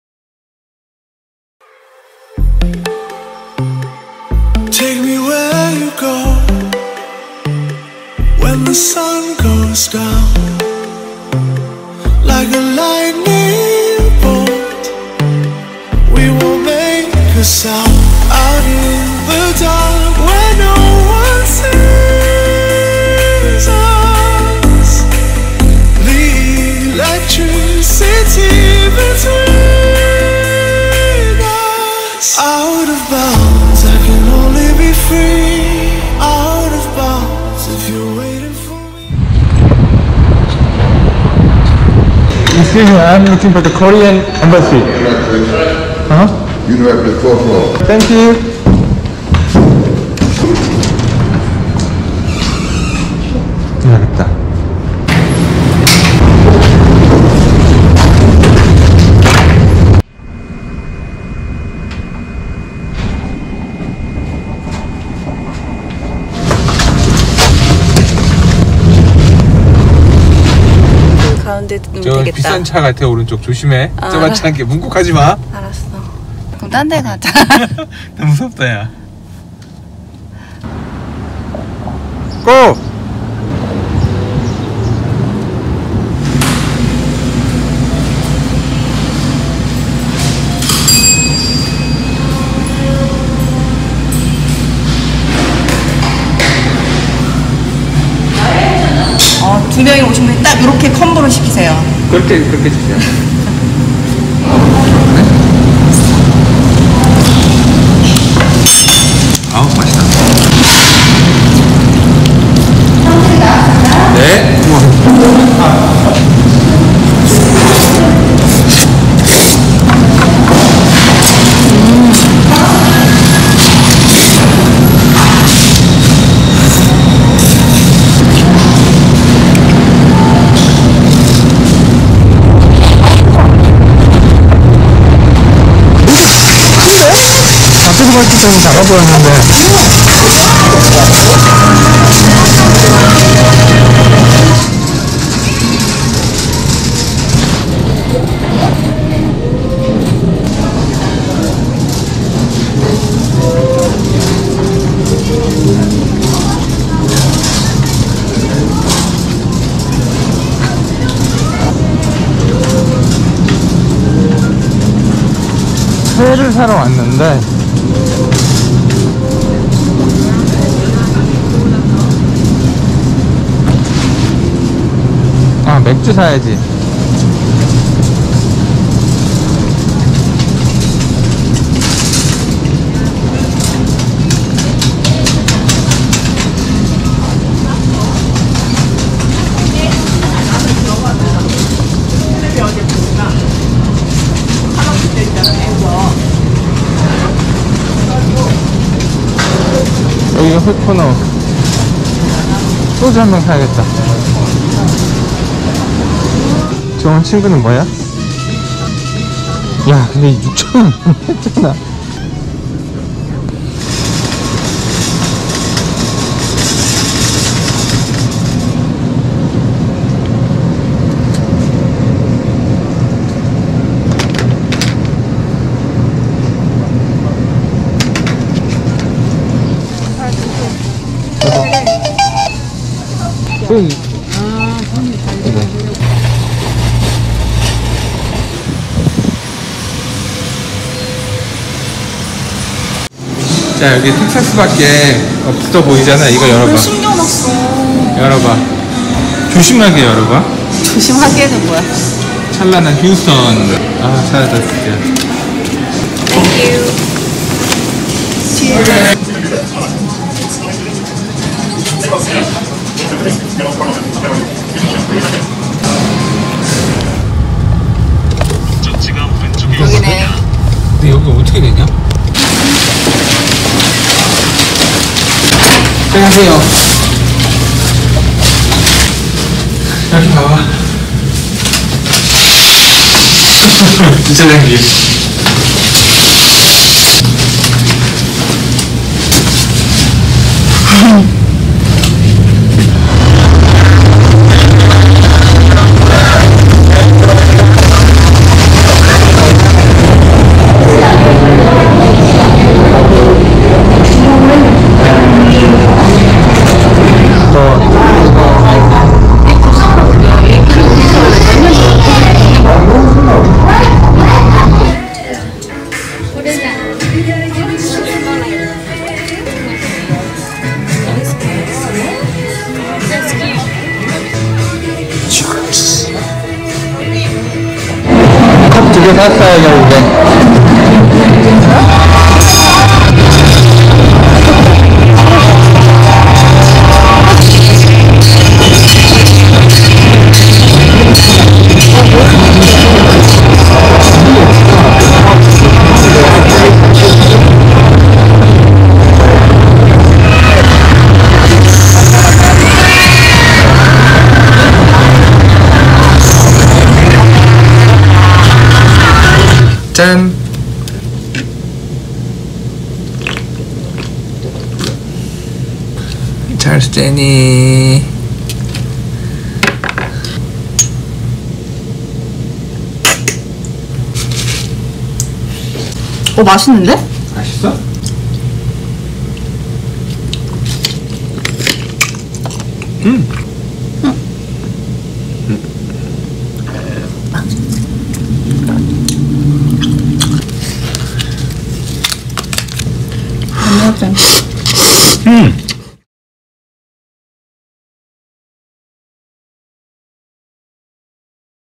[🇺🇸미국여행] 텍사스 휴스턴 영사관, 한인타운 나들이 Houston Korea town, HMart
💡설정에서 고화질로 감상 가능해요^^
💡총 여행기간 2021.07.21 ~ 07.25